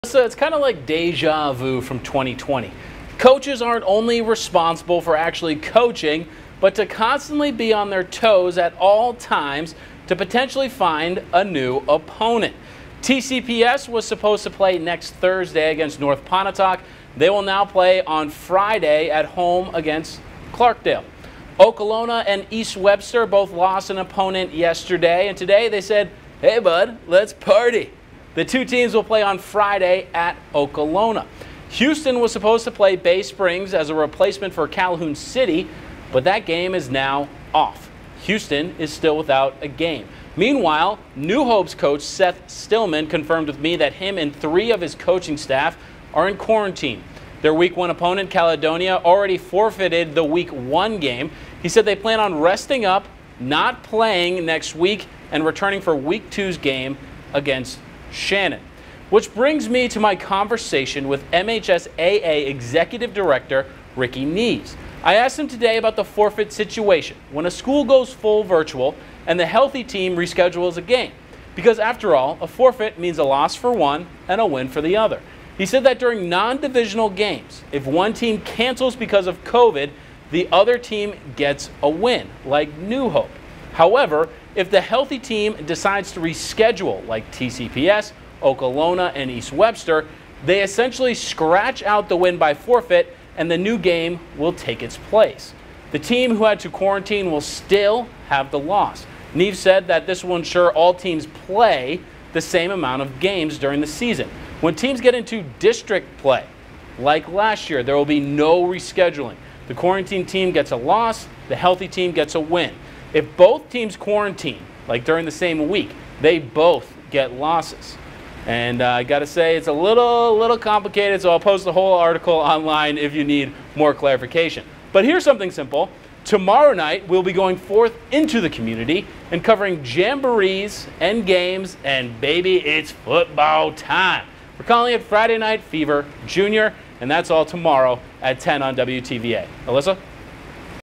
Several teams update schedule for Week 1
TCPS, Okolona, and East Webster all rescheduled games as well as an explanation of the forfeiting rules